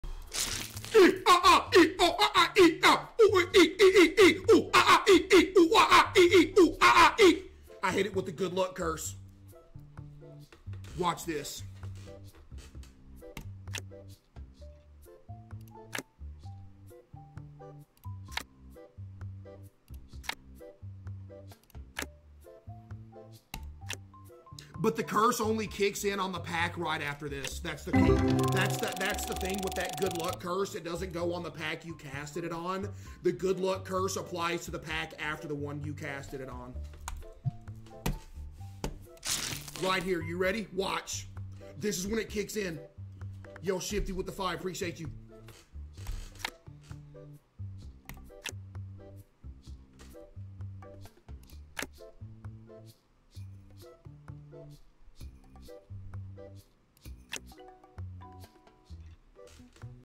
Case Uses The Good Luck Curse (It Didn't Work)
Visit CaseOh on Twitch for more funny content.